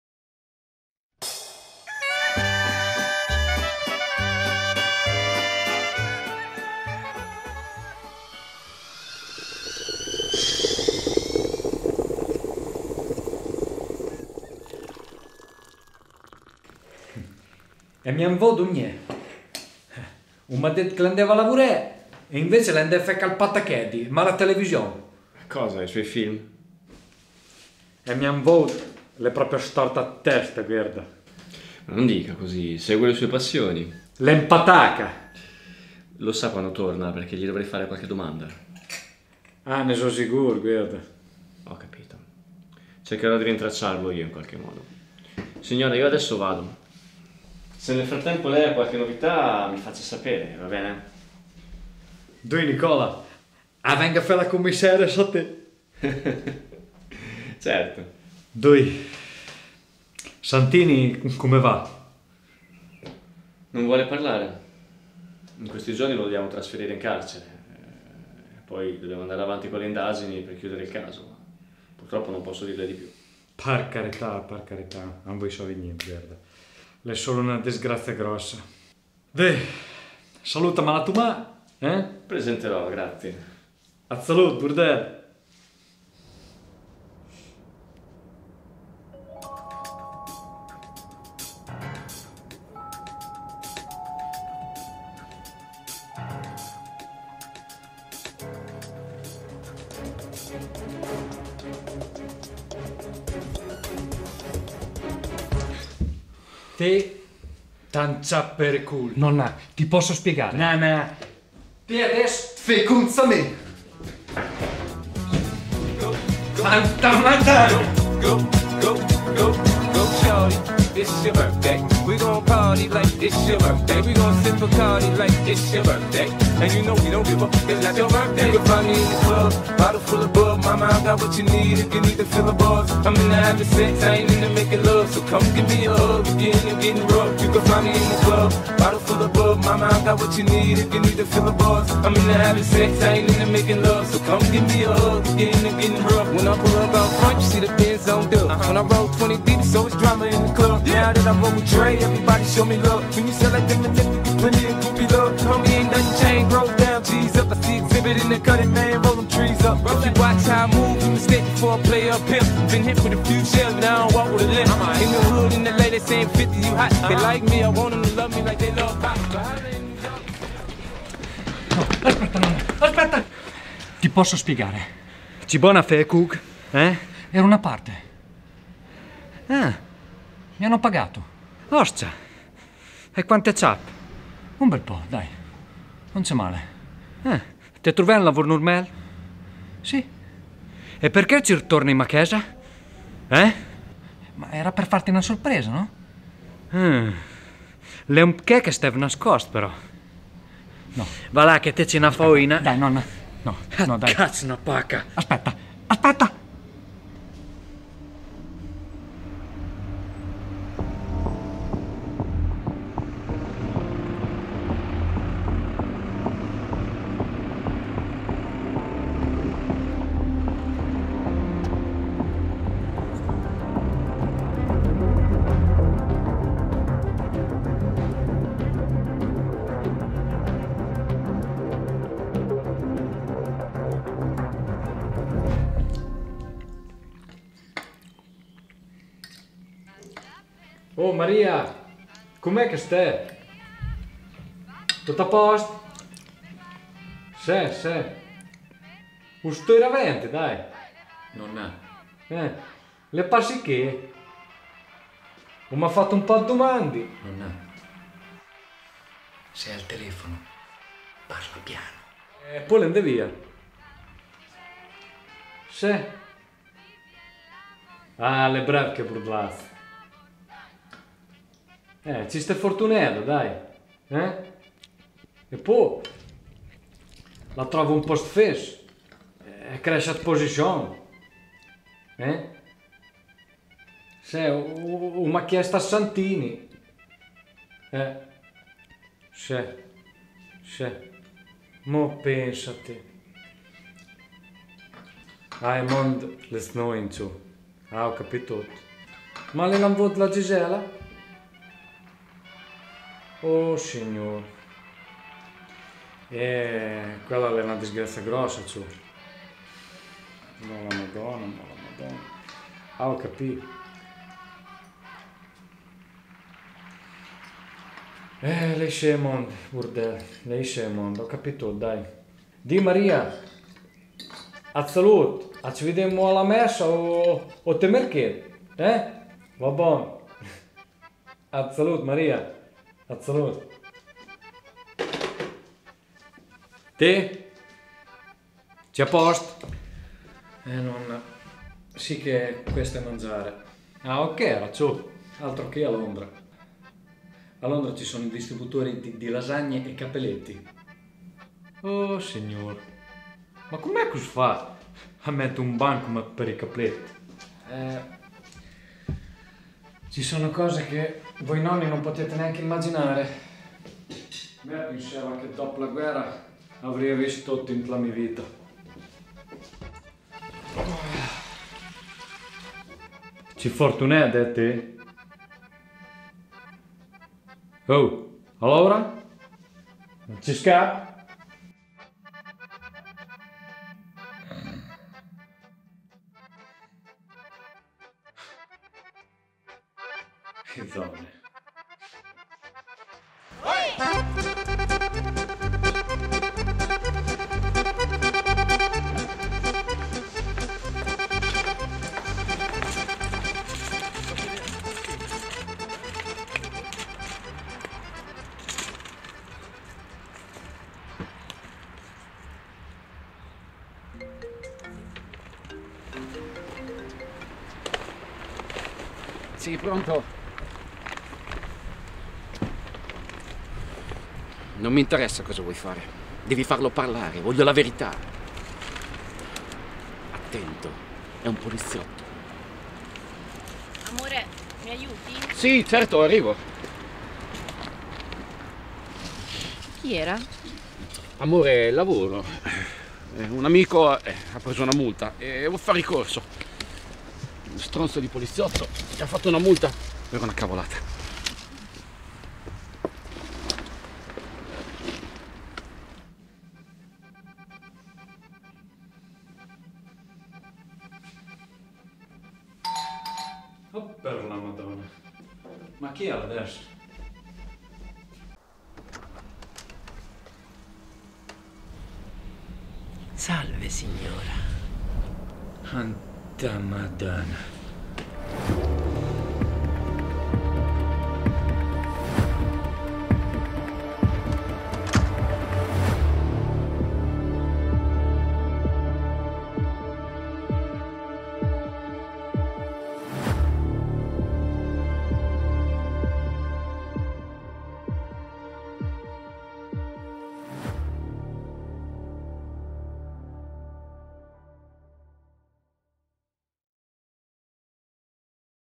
"LA SUPERNONNA" EPISODIO 5
Nicola diventando commissario e prendendo le redini di Santini continua ad indagare sul caso per avere chiarezze, cerca di fare domande persino al nipote.
Nel frattempo il nipote ritorna a casa dai suoi viaggi, la Nonna si accorge della sua comparsa come cuoco nei film, lei si arrabbia, dopo una piccola lite cercano di perdonarsi a vicenda.
La Nonna chiama la sua amica Maria madre di Nicola, gli dice che Nicola è venuto a chiedere del Nipote e la Maria gli racconta la ex vita di Santini.
Il capo della Banda Freselli mentre è al supermercato con la moglie riceve una telefonata dal suo scagnozzo che ha in mente una cosa spietata,
quindi cosa succederà alla Nonna ???

Scritto e Diretto da Michel Rattini
Cast :  Nicola Pagliarani, Luca Bartolini, Rosita Della Chiesa, Simone Ferri,  Mattia Arlotti, Michel Rattini.